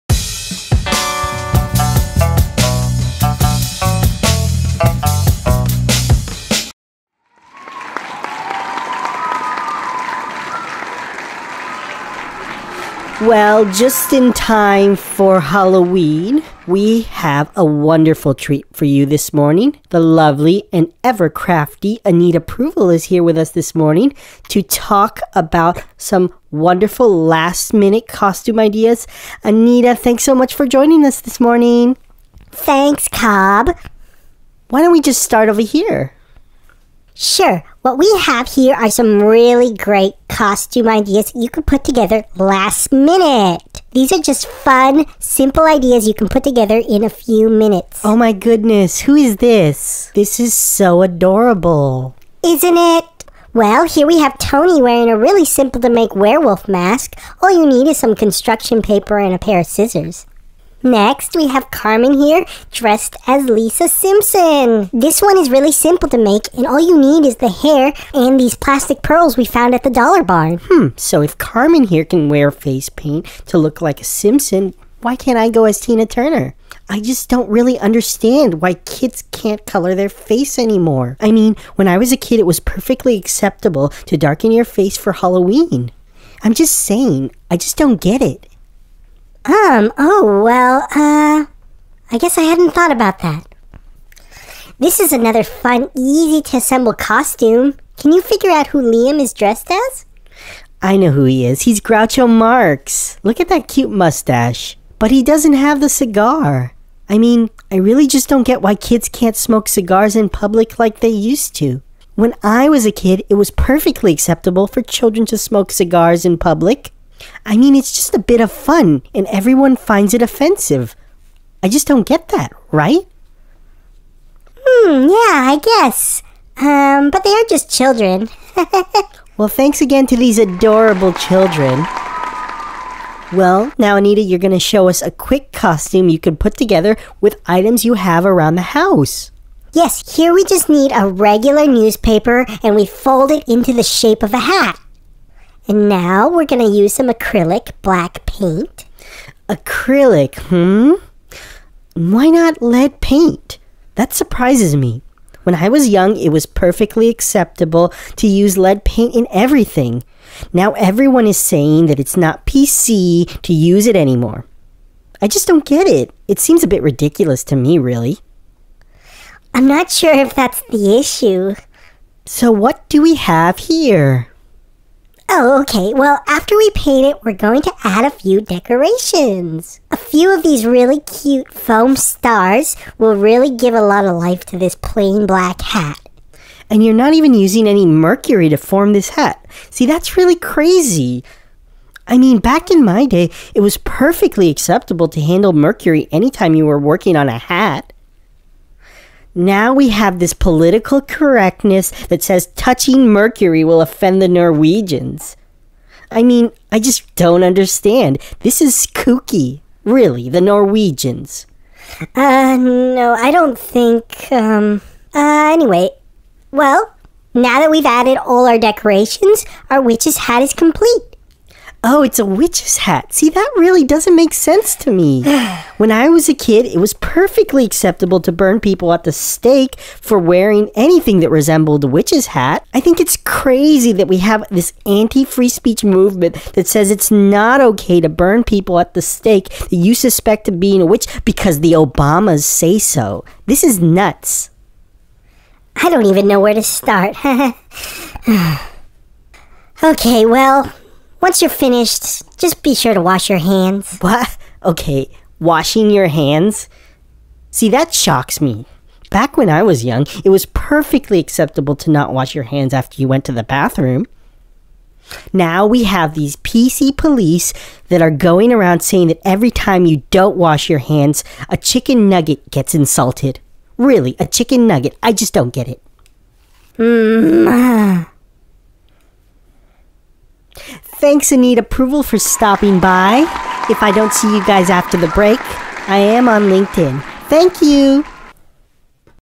Last Minute Halloween Costume Ideas | NERDUMB
Cob Forbrains wonders why things aren't like they used to be in this final Halloween addition of Cobbler Forbrains Today. Be sure to watch Cob on Celebrity Big Brother and Dancing With The Stars as she tries to resurrect her career.